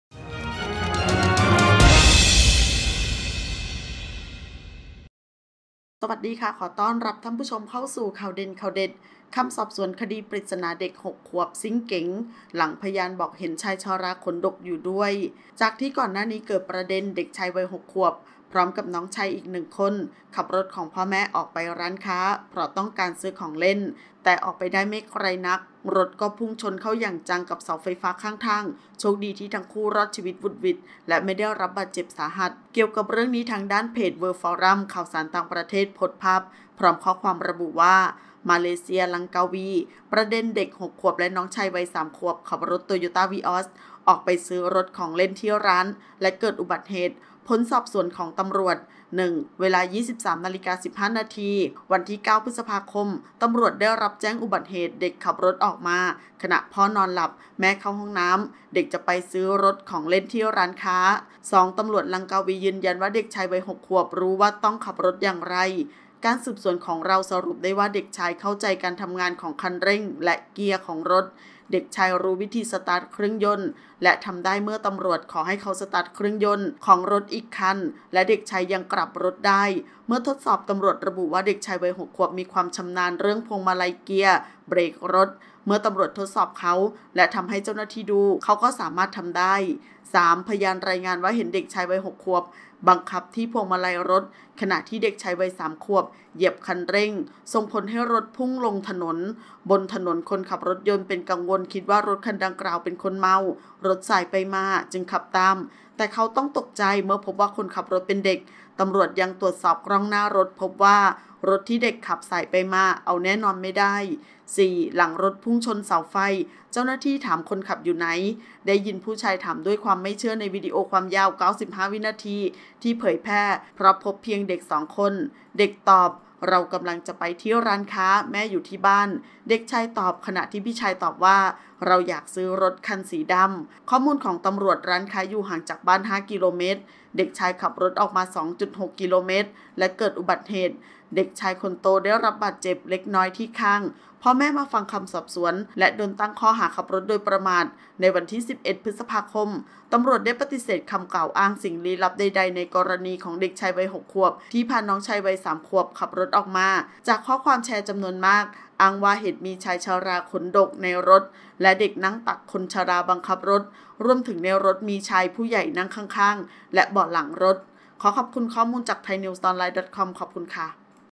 คำสอบสวนคดีปริศนา เด็ก 6 ขวบซิ่งเก๋ง หลังพยานบอกเห็นชายชราขนดกอยู่ด้วย
00:00 - Intro
00:10 - News
01:00 - Minute